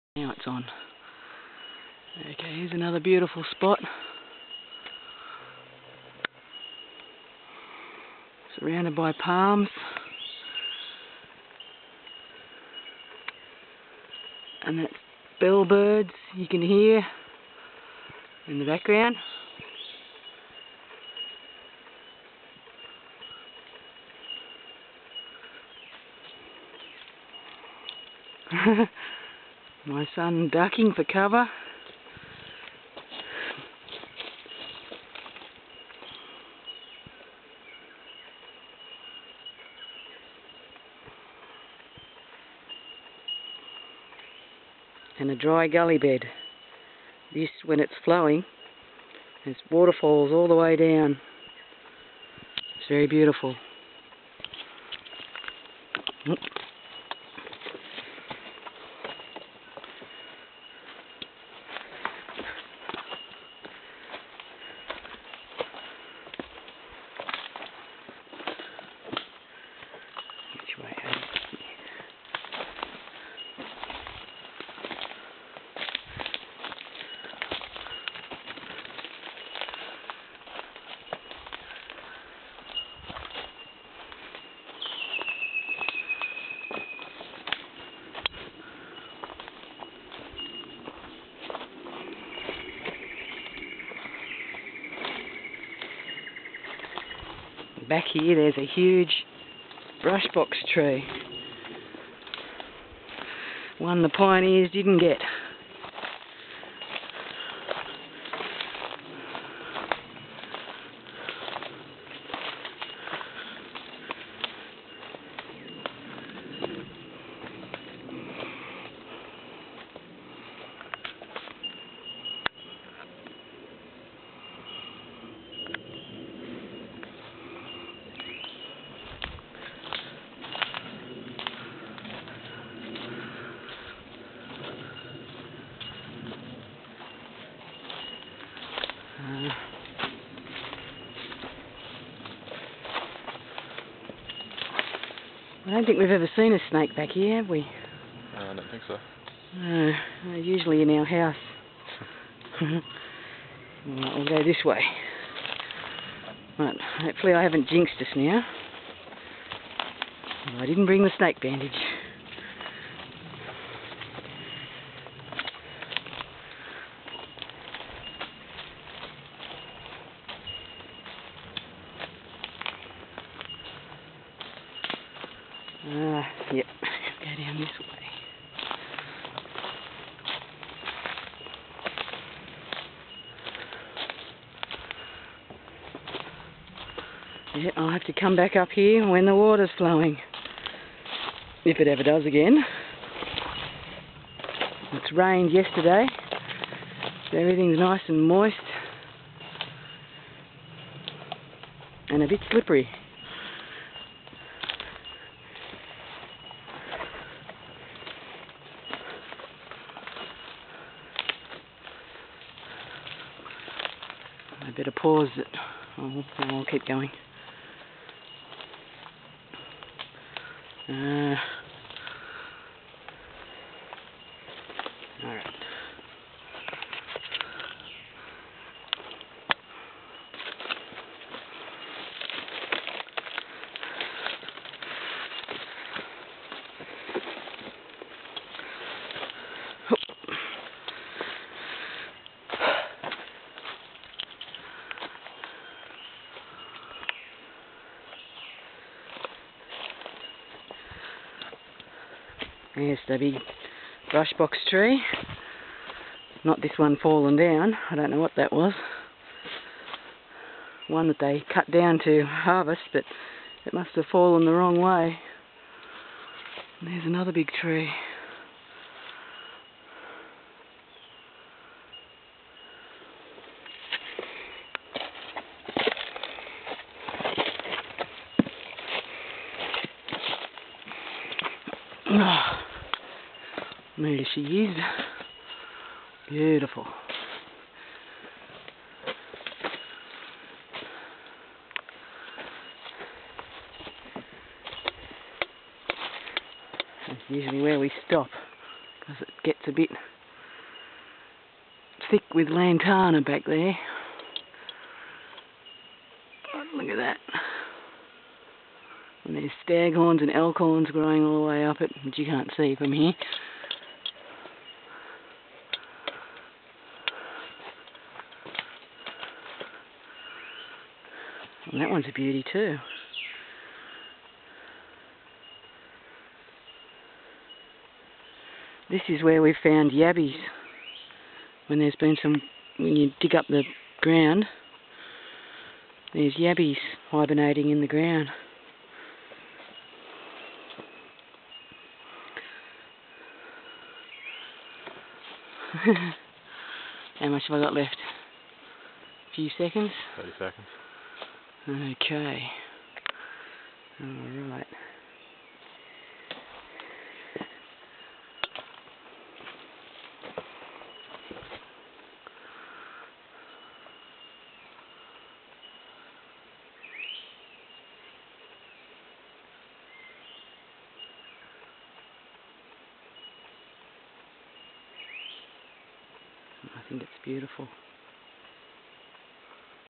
Goin' for a walk in our rainforest gully
My son and I going for a morning walk in our rainforest gully in NSW Australia